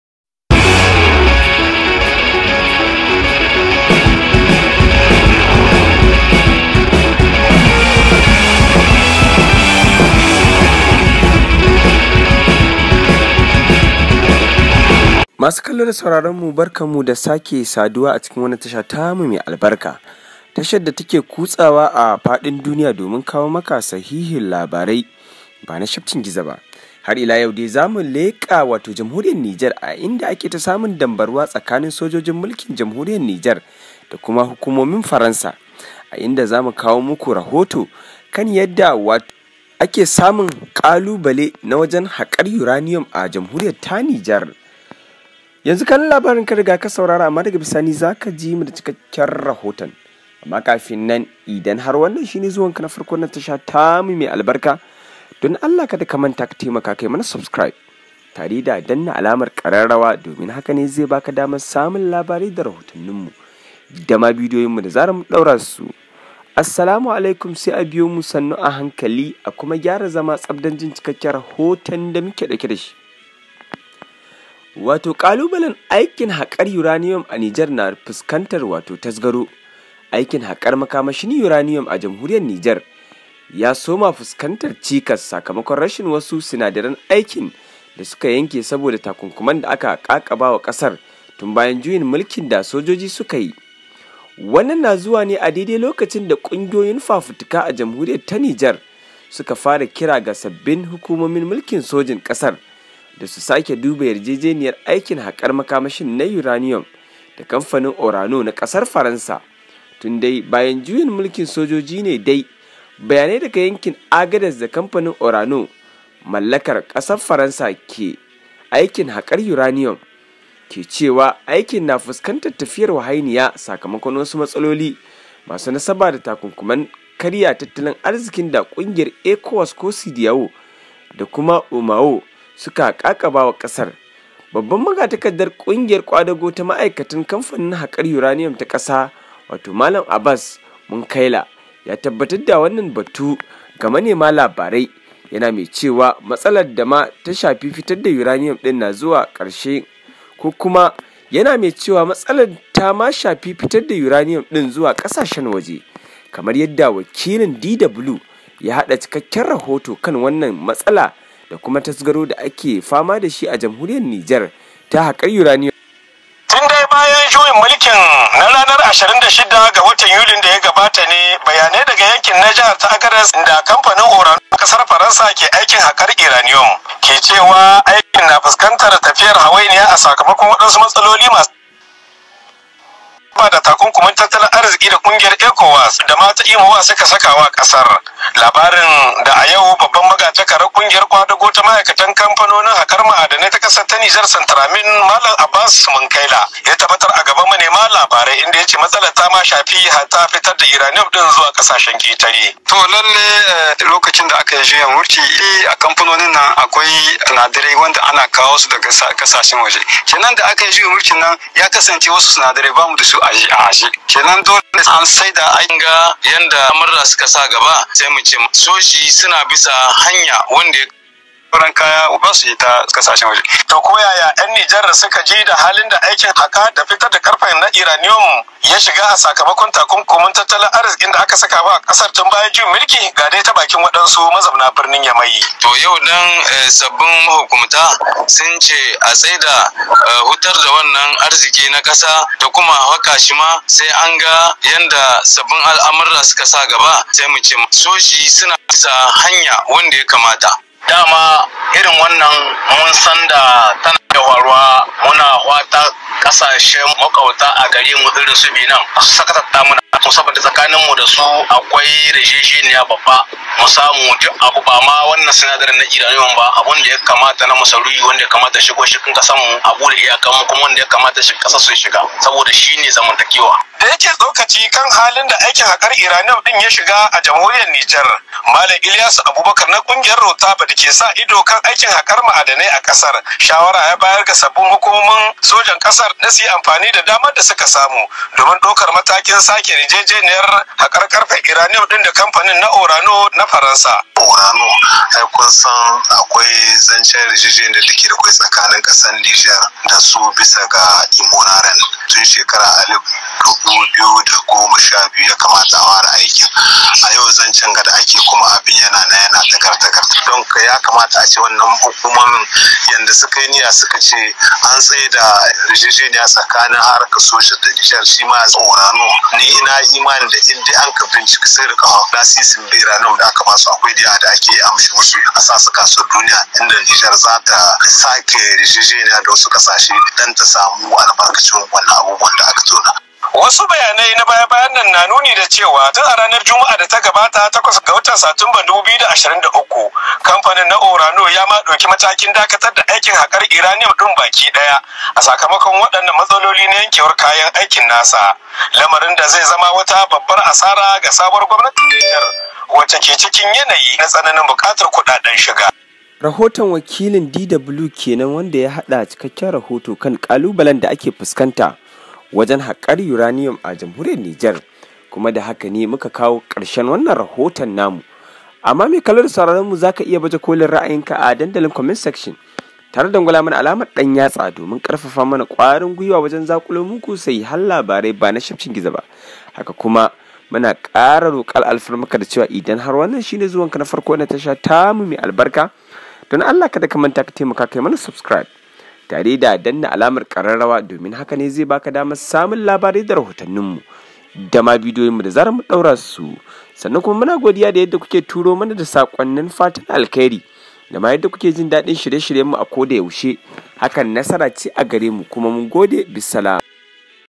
JUYIN MULKIN NIJAR: KAMFANIN FARANSA ORANO NA HAQAR YURANIYUM YA BAR NIJAR KO AKAWO WASU KAMFANUNUWA
Dagin Arewa TV